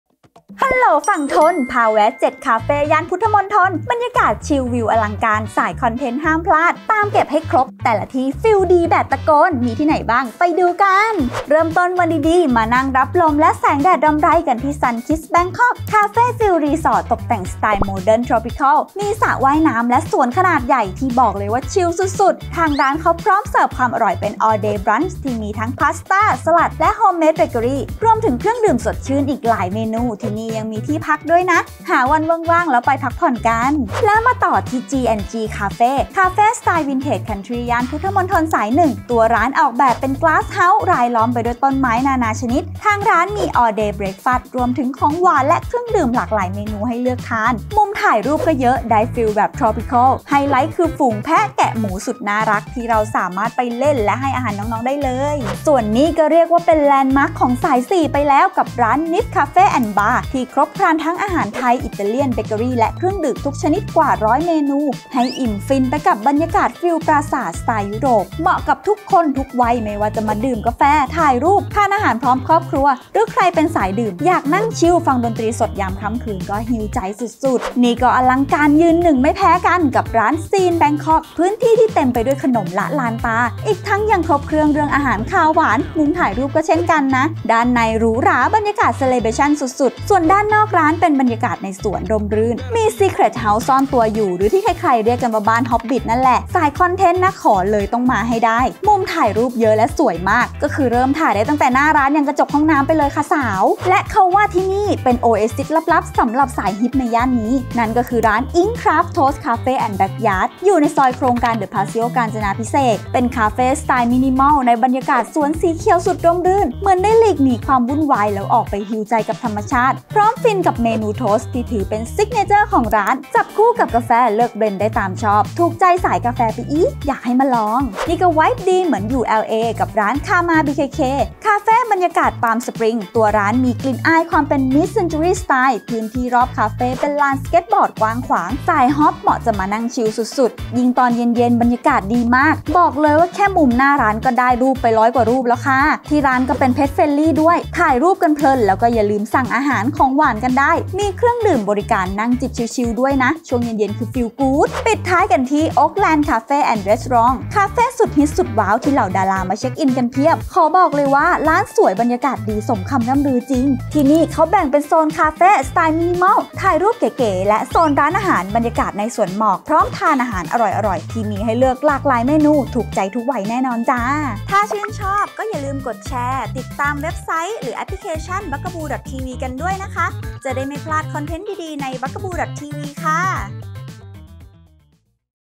บอกต่อ 7 คาเฟ่ย่านพุทธมณฑล บรรยากาศชิล วิวถ่ายรูปปัง | Bugaboo Cafe | Ep.30
สายคาเฟ่เค้ารู้กัน คาเฟ่พุทธมณฑล ยืนหนึ่งเรื่องความสวยชิค มุมถ่ายรูปปัง Cafe Hopper คนไหนยังไม่เคยแวะมาคาเฟ่ฝั่งธน ลองเปลี่ยนบรรยากาศมาเที่ยวคาเฟ่ชานเมืองกันบ้างดีกว่า กับ 7 คาเฟ่ย่านพุทธมณฑล บรรยากาศชิล วิวถ่ายรูปปัง รับรองแต่ละที่ถูกใจสายคาเฟ่อย่างเรา ๆ แน่นอน

1. SUNKISS.bangkok • พุทธมณฑลสาย 1
2. G&G Cafe • พุทธมณฑลสาย 1
3. Nives Cafe and Bar • พุทธมณฑลสาย 4
4. Scene Bangkok • พุทธมณฑลสาย 4
5. Ing Craft Toast Cafe & Backyard • พุทธมณฑลสาย 2
6. Karma bkk • พุทธมณฑลสาย 4
7. Oakland Cafe x Restaurant • พุทธมณฑลสาย 1

ดูรายละเอียดและการเดินทาง